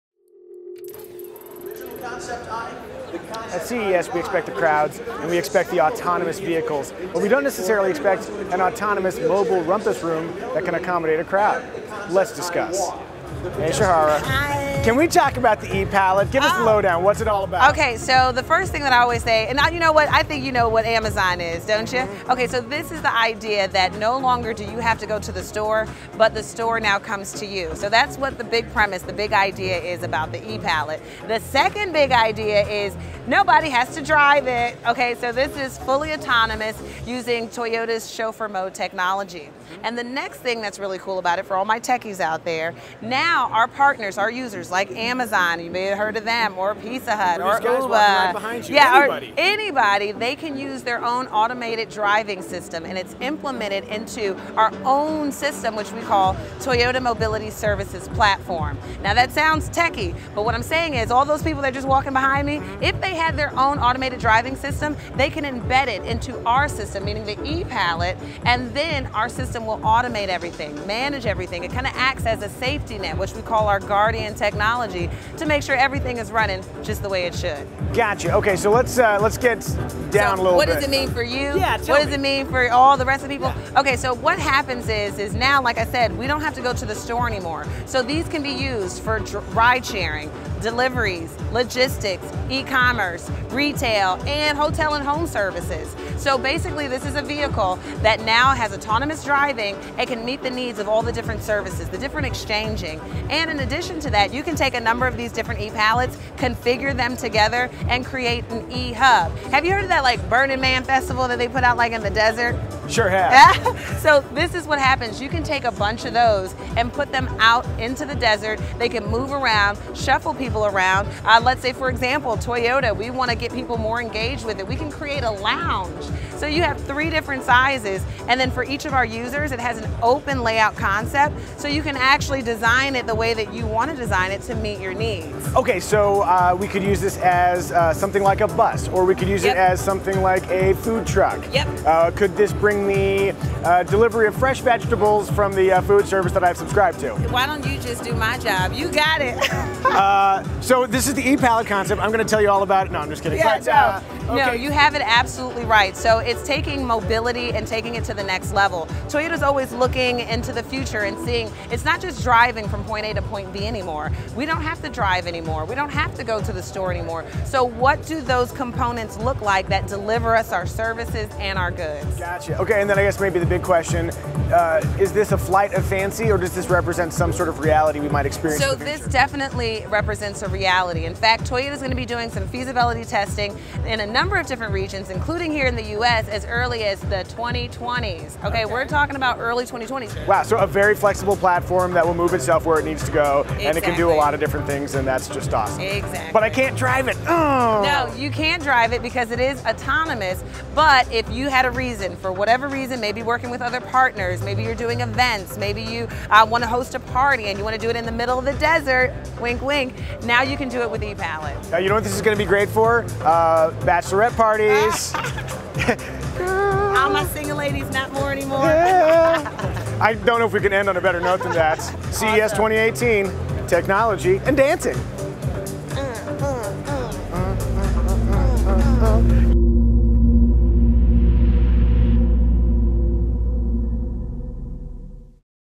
Toyota e-Palette Concept - CES 2018
Kelley Blue Book's coverage of the Toyota e-Palette Concept at CES 2018.

Toyota envisions the e-Palette concept as an open-source mobility solution for varied tasks ranging from delivery services, ride sharing, and retail shopping. In this video Kelley Blue Book's Micah Muzio speaks with a Toyota product expert to learn when and how e-Palette might revolutionize the field of mobility.

Kelley Blue Book is your source for new car reviews, auto show coverage, features, and comparison tests.